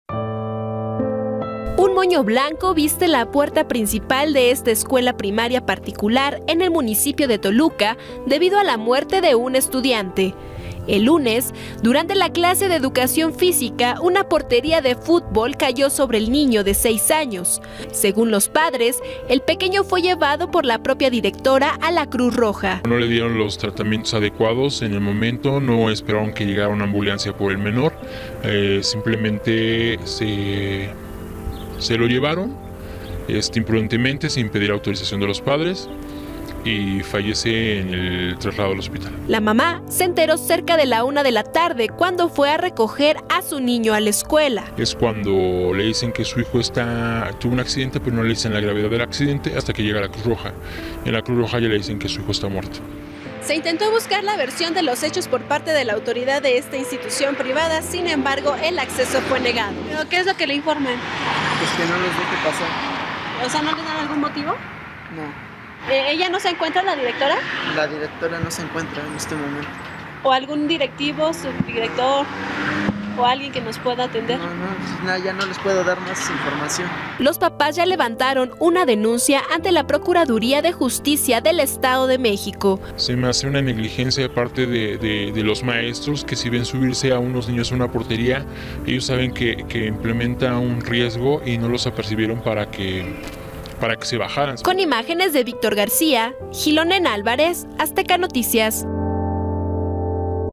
Muere menor tras accidente en escuela | Noticias del Estado de México
Un menor de 6 años de edad perdió la vida en una escuela primaria particular del municipio de Toluca, luego de que el pasado lunes en la clase de educación física, le cayera una portería de futbol.